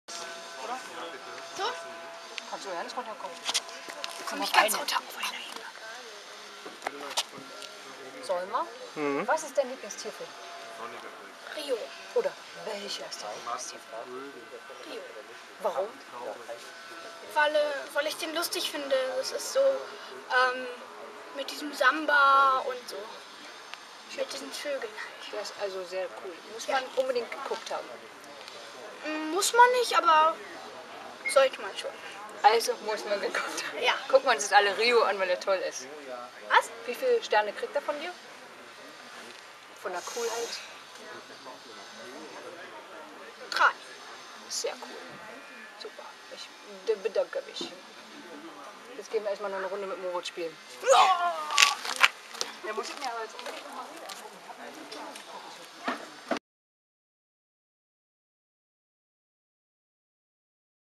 Filmtiervoting: Max Boekhoff mag den Film Rio
Max Boekhoff hat eine große Karriere vor sich. Nachdem er neben meinem Schwein Moritz die Hauptrolle im Sat.1 Film "Was Tiere wollen" gespielt hat, muss das ja so sein :). Auch er hat einen Lieblingstierfilm. Und der heißt RIO. Wie heißt Dein Lieblingsfilm?